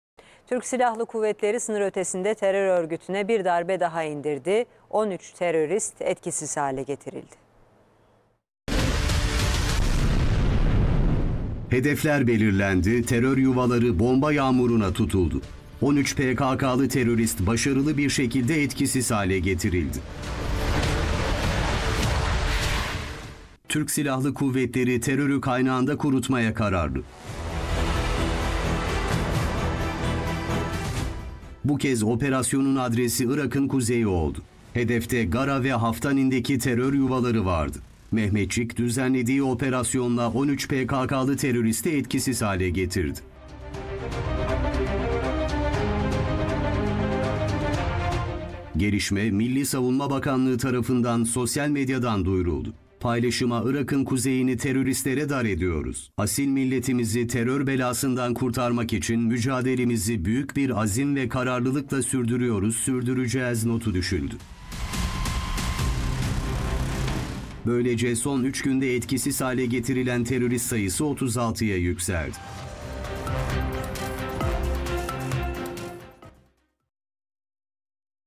MSB duyurdu! Irak'ın kuzeyinde 13 PKK'lı terörist öldürüldü | A Haber
Milli Savunma Bakanlığı, Irak'ın kuzeyinde 13 PKK'lı teröristin hava harekatıyla etkisiz hale getirildiğini bildirdi.

Bakanlıktan yapılan açıklamada, şu ifadelere yer verildi:

"Irak'ın kuzeyini teröristlere dar ediyoruz. Kahraman Türk Silahlı Kuvvetlerimiz, Gara ve Haftanin bölgelerinde tespit ettiği 13 PKK'lı teröristi düzenlediği hava harekatıyla etkisiz hale getirdi. Asil milletimizi terör belasından kurtarmak için mücadelemizi büyük bir azim ve kararlılıkla sürdürüyoruz, sürdüreceğiz."